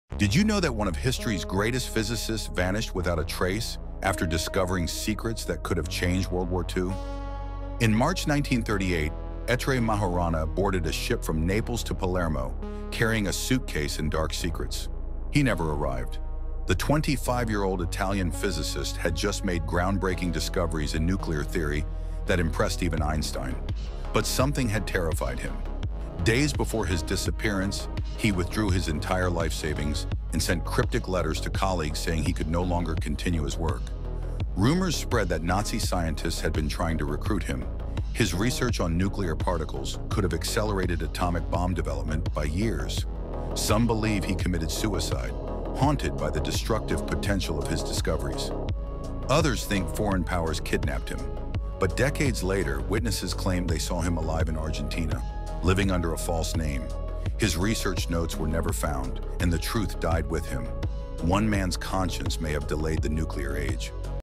The Physicist Who Vanished After Discovering Nuclear Secrets That Could Change WWII
In 1938, one of the world’s most brilliant minds — Ettore Majorana, an Italian physicist hailed as a genius equal to Einstein — boarded a ship bound for Naples… and was never seen again.

He had just uncovered nuclear secrets that could have changed the course of World War II — breakthroughs in particle physics decades ahead of his time. Then, suddenly, he vanished without a trace.

Was he kidnapped? Did he defect? Or did he fake his own death to stop his discovery from becoming a weapon?

In this video, we uncover the mystery behind Majorana’s disappearance, his connection to early atomic research, and the shocking theories about what he may have known — and why powerful forces might have wanted him gone.